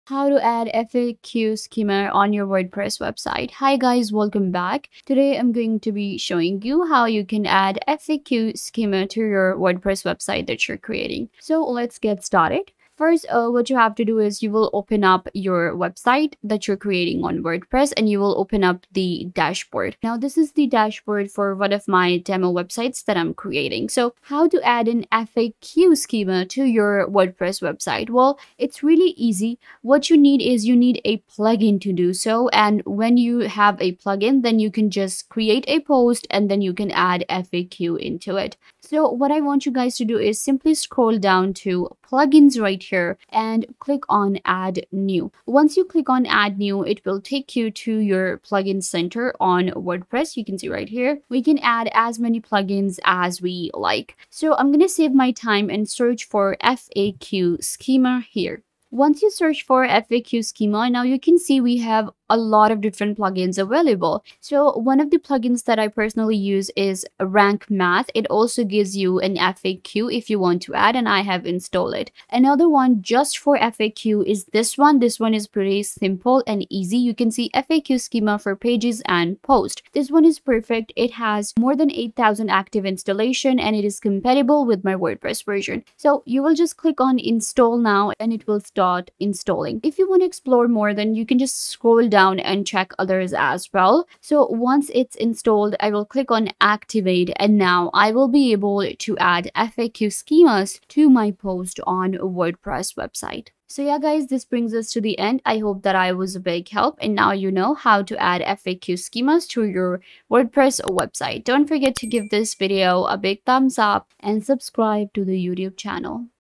How To Add FAQ Schema In Wordpress 2026 (EASY WAY)
In this video I show you How To Add FAQ Schema In Wordpress. It is really easy to do and learn to do it in just a few minutes by following this super helpful tutorial.